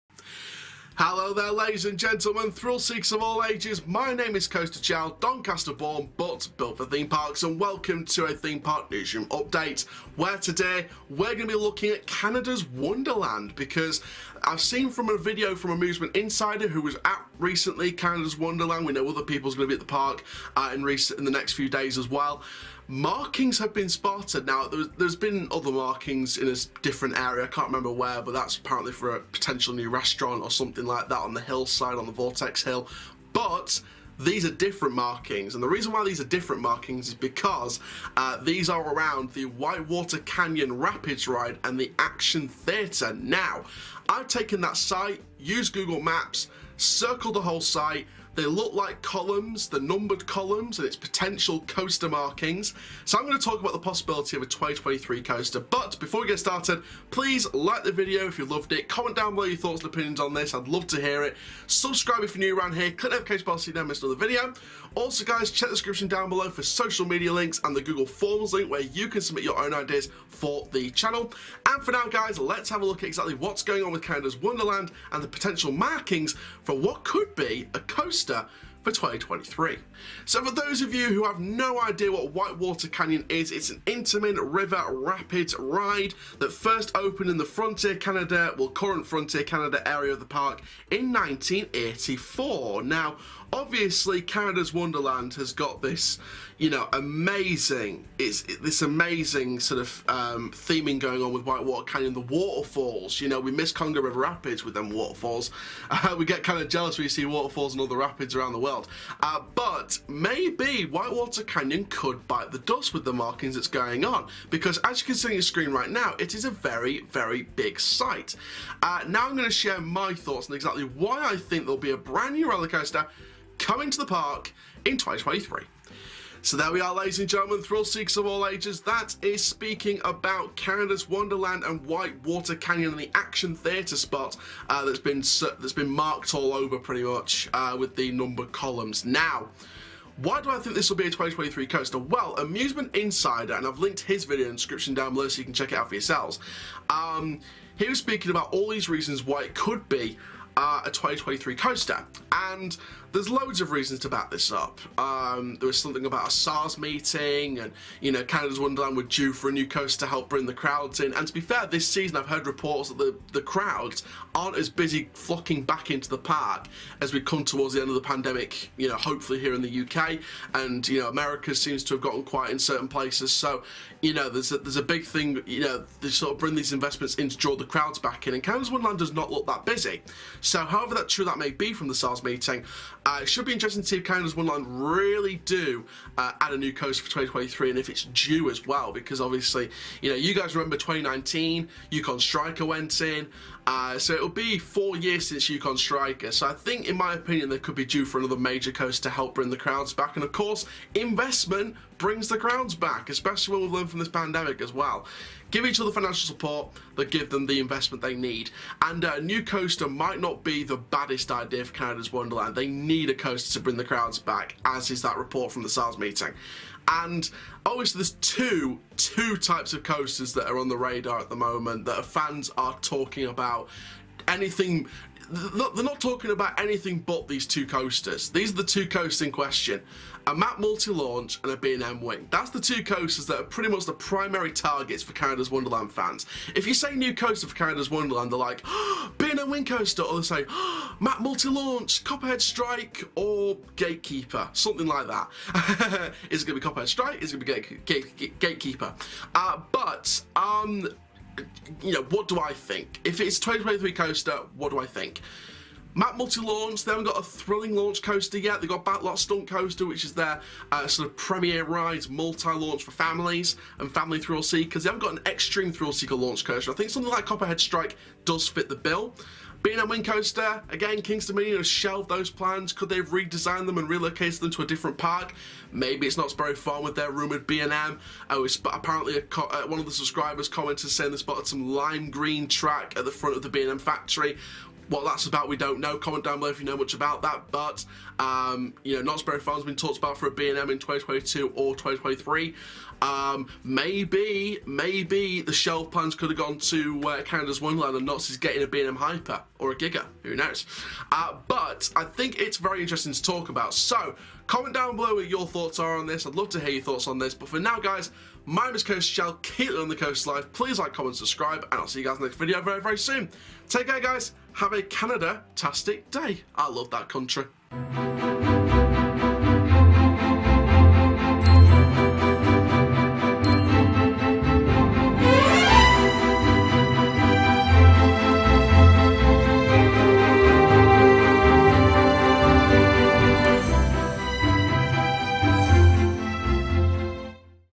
Markers Are New Around White Water Canyon At Canada's Wonderland Which Could Mean A 2023 Coaster
On this episode of Theme Park Newsroom, I'll be sharing exciting information on Canada's Wonderland potential new rollercoaster for the 2023 season!

Intro: 0:00
Facts and Stats: 1:19
My Thoughts: 2:05
Outro: 5:17